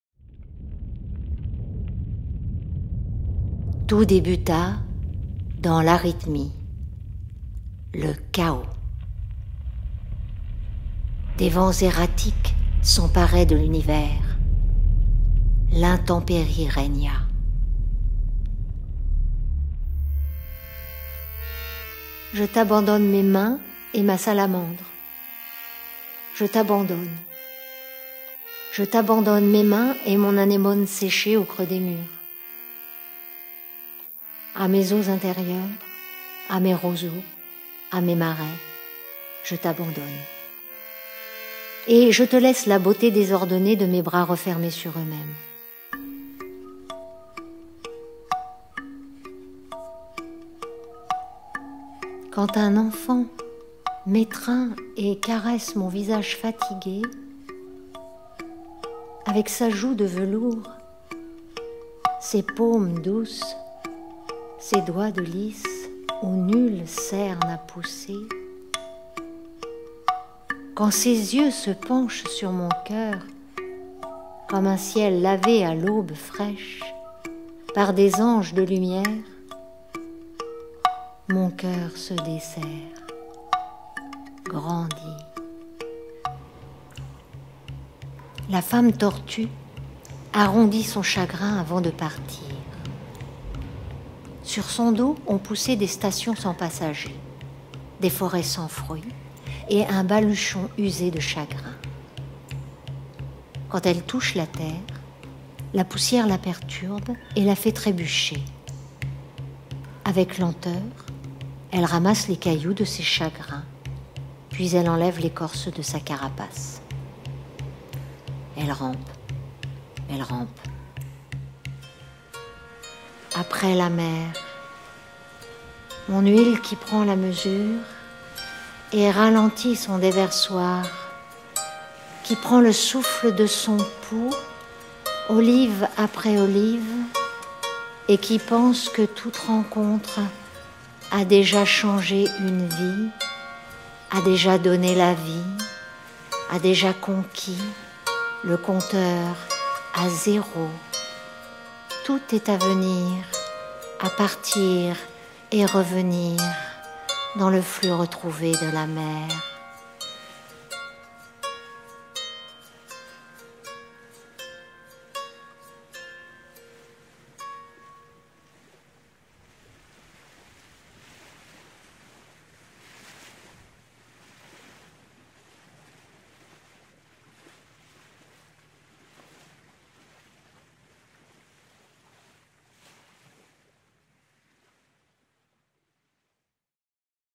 Après la mer
Le spectateur est invité à un voyage sonore.
Une bande-son l’embarque à travers différentes ambiances de nature :
ruisseau, vent, insectes, oiseaux, ressac…
Surgit la voix de la comédienne, nous guidant dans les univers poétiques de
Andrée Chedid, Maram al-Masri, Lâle Müldür,
Samira Negrouche, Nadia Tueni, Fadwa Touqan …
Le musicien ponctue cette lecture avec des instruments acoustiques,
faisant écho à la poésie de ces femmes d’hier et d’aujourd’hui